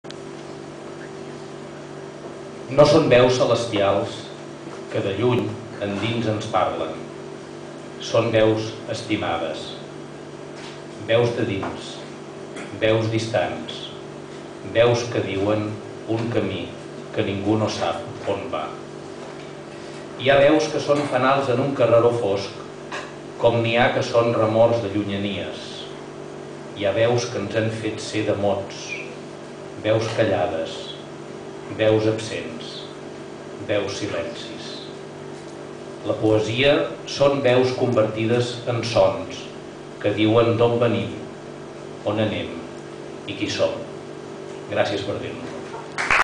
Poefesta a l'Horiginal BCN [2]
Homenatge a Josep Piera, al Pub l'Horiginal de Barcelona